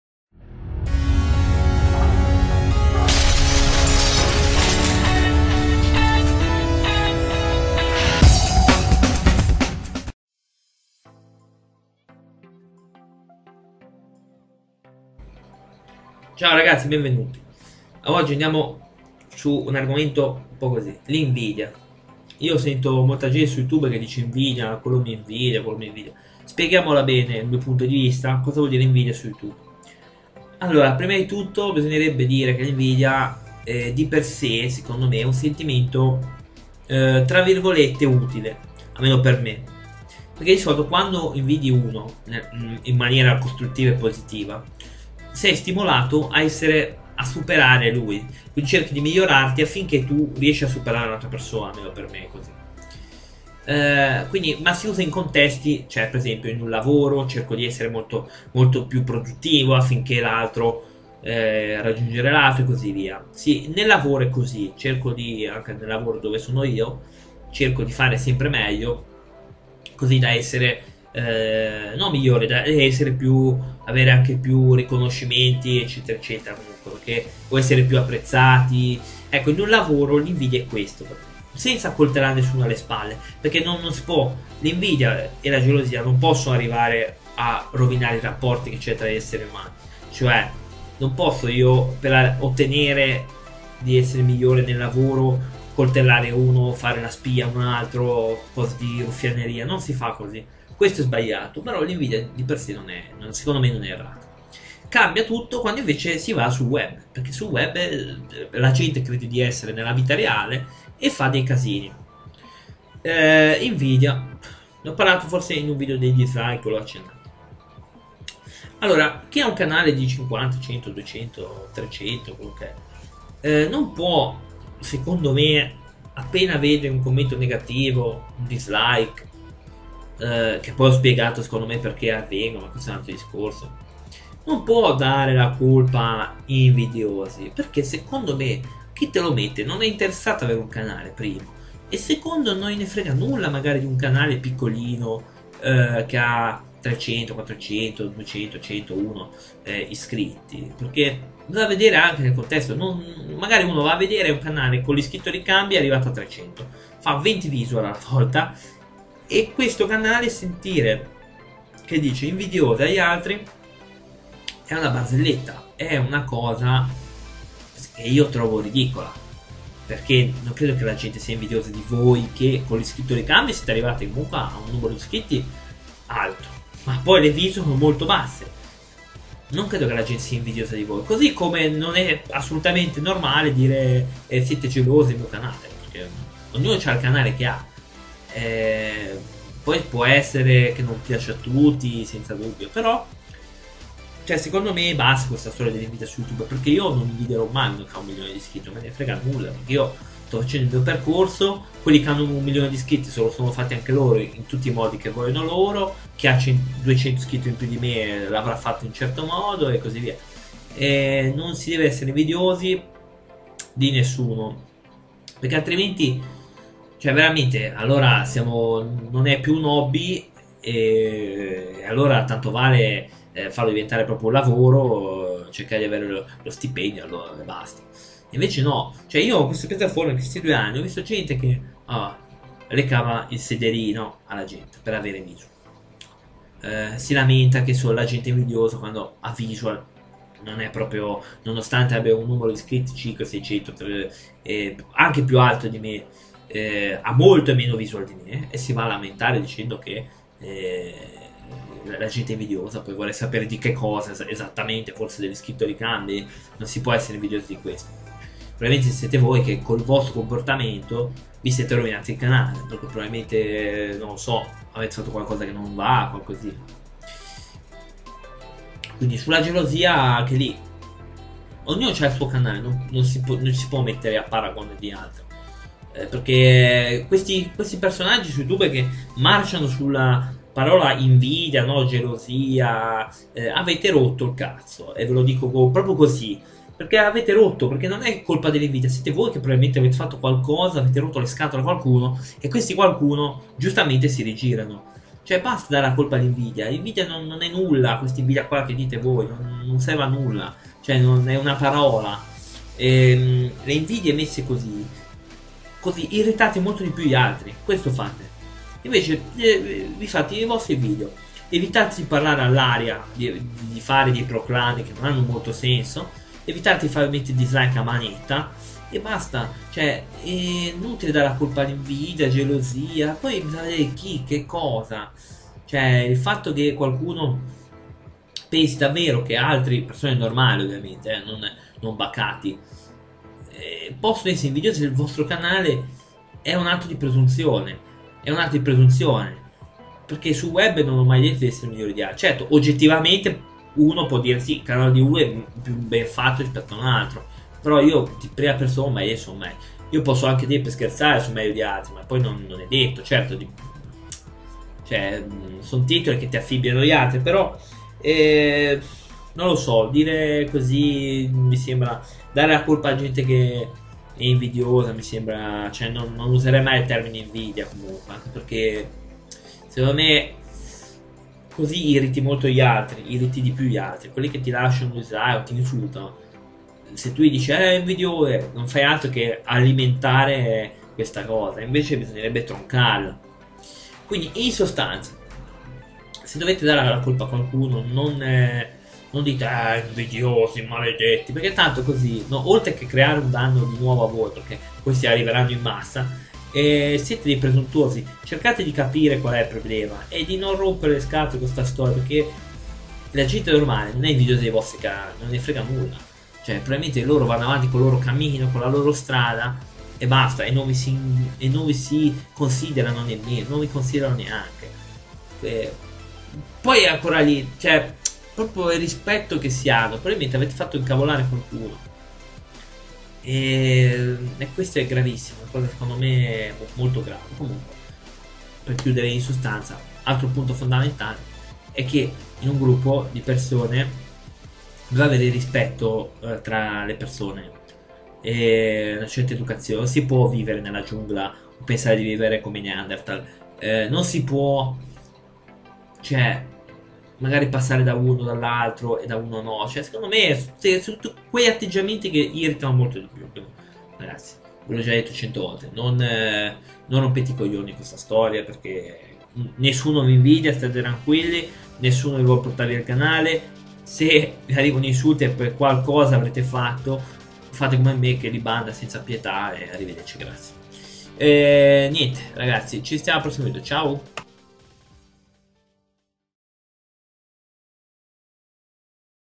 Invidia su youtube, ma ci credete davvero?
Questa è una riposta a chi si crede davvero che  il proprio canale venga invidiato da tutti:
Ma pensate davvero tutti siano invidiosi del vostro canale?
youtube è un hobby,svegliatevi e non rompete le scatole..

Questi sono i miei indirizzi:
Volete offrirmi un caffè? venite qui u.u